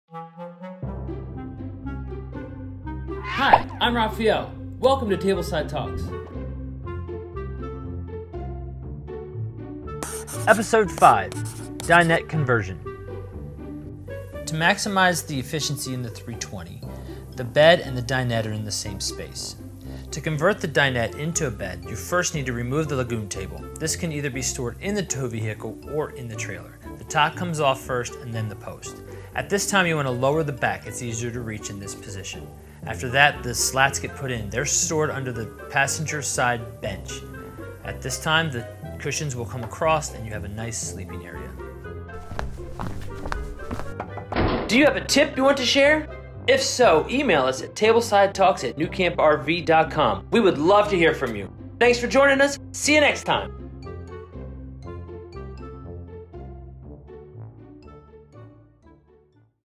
Going from Table to Bed in Your TAB
Have you ever wondered how easy it is to turn the TAB table into a bed?

We want to help you maximize every inch of space in your camper so our latest Table Side Talk shows you exactly how to turn your dinette area into a queen-sized bed!